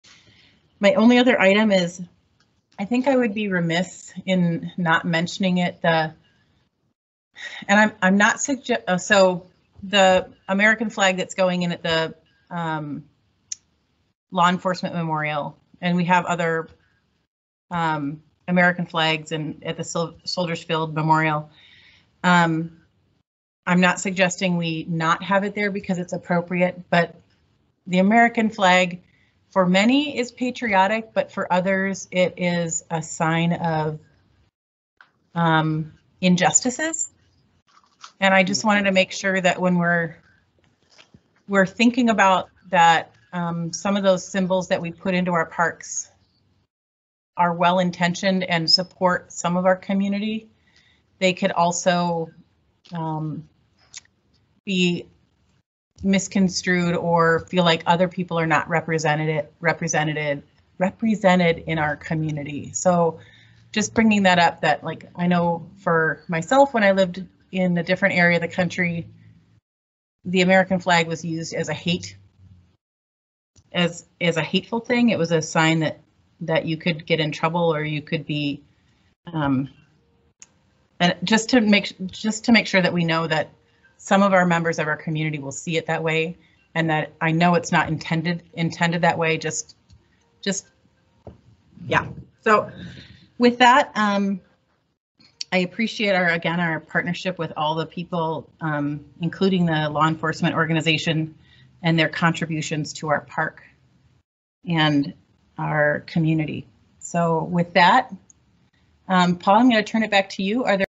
RochMN Parks & Rec Pres: American Flag is a "Sign of Injustices"
SOUND OFF: What do you think about Archer's comments about displaying the American flag in our parks?

During the Rochester Parks and Recreation Board meeting on April 6, 2021, Board President Linnea Archer expressed her concerns about displaying the American flag at the Law Enforcement Memorial that is being constructed at Soldiers Field Park. "The American flag for many is patriotic but for others, it is a sign of injustices," Archer said.  She also said that in her experience the American flag has been "used as a hateful thing, it was a sign that you could get in trouble..." and that some people in the Rochester community will also see it that way. Archer said she is not suggesting that the American flag should not be displayed in our parks, but it could be seen differently by some. "When we are thinking about some of those symbols that we put in our parks are well-intentioned and support some of our community, they can also be misconstrued or feel like other people are not represented in our community."